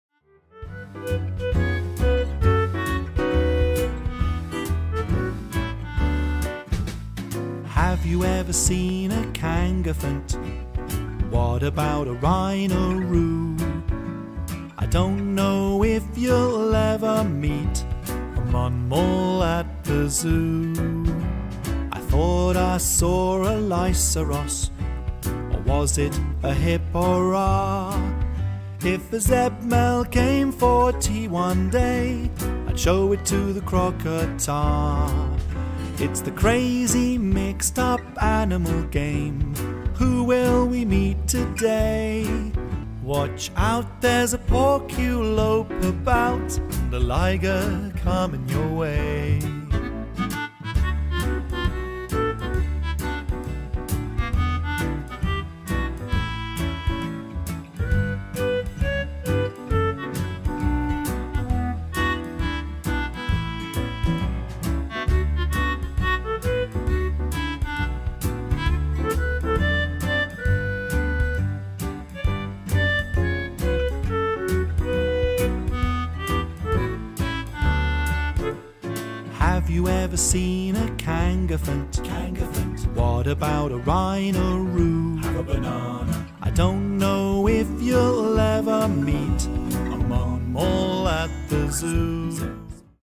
The Crazy Mixed Up Animal Game
During my nursery sessions this week, we've been showing how we brush our teeth then took a drive in our big truck to the see the crazy animals before rowing with our friends down the lazy river.

We always love singing and dancing, but we also have an opportunity to learn a little bit.  We talked about the animal names, the sound each animal makes, their shape and what colour the children needed, to make the animal match (or crazy if they preferred).  It was also lovely to watch their fascination of the how the magnets work.